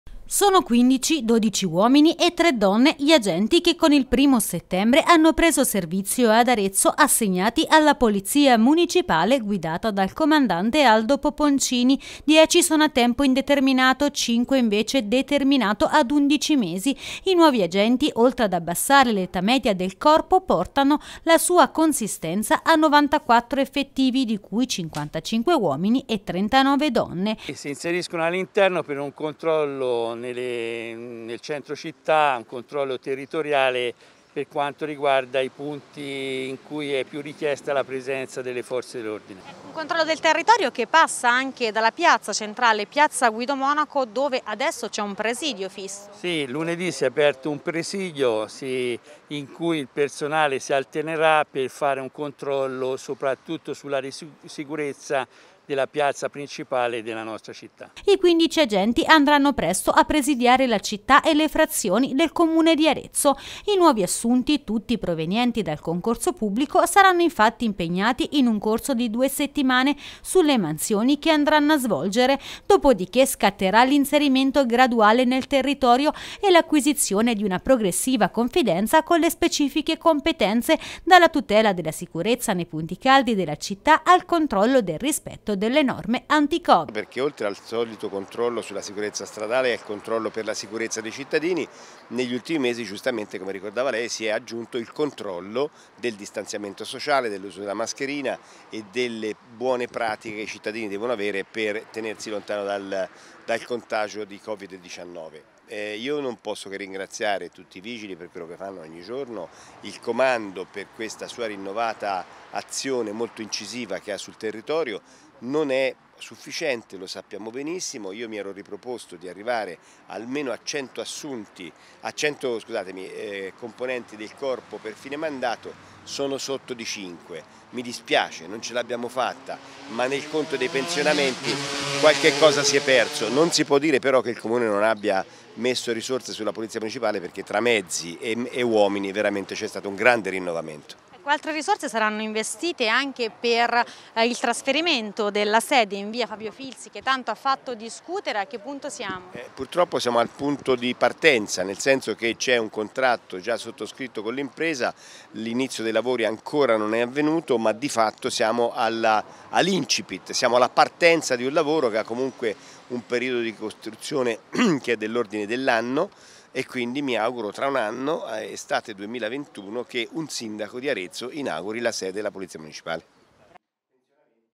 Polizia Municipale, ad Arezzo arrivano 15 nuovi agenti
Sono 15, 12 uomini e 3 donne, gli agenti che con il primo settembre prendono servizio ad Arezzo, assegnati alla polizia municipale guidata dal comandante Aldo Poponcini.

Sono 10 a tempo indeterminato e 5 a tempo determinato a 11 mesi. Oltre ad abbassare l’età media del corpo, portano la sua consistenza a 94 effettivi, di cui 55 uomini e 39 donne.

È stato il giorno della foto di gruppo e dei primi auguri di buon lavoro. Da domani, i nuovi assunti, tutti provenienti da concorso pubblico, saranno impegnati in un corso di due settimane sulle mansioni che andranno a svolgere. Dopo di che, scatterà l’inserimento graduale nel territorio e l’acquisizione di una progressiva confidenza con le specifiche competenze, dalla tutela della sicurezza nei punti “caldi” della città al controllo del rispetto delle norme anti-covid.